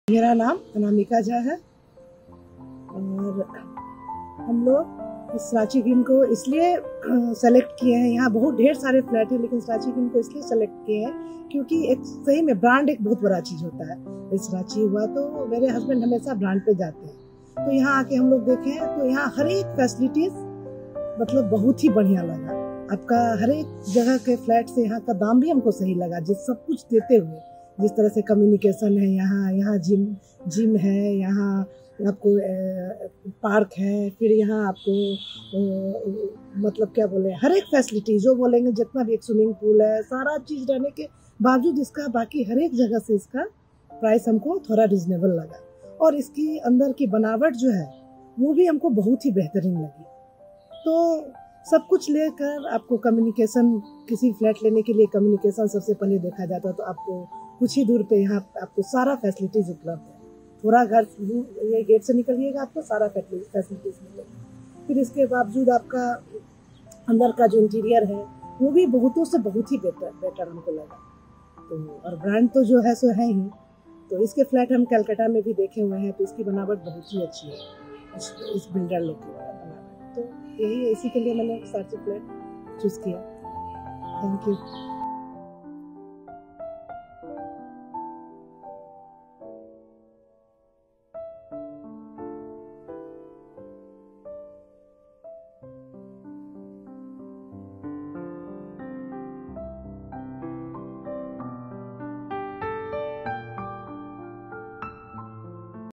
Shrachi Greens Customer Testimonial- Anamika Jha
Proud home buyer gives us heartfelt insight about her choice of Shrachi Greens, Jamshedpur.𝐋𝐈𝐕𝐄 𝐘𝐎𝐔𝐑 𝐃𝐑𝐄𝐀𝐌𝐒 𝐖𝐈𝐓𝐇 𝐒𝐇𝐑𝐀𝐂𝐇𝐈 𝐆𝐑𝐄𝐄𝐍𝐒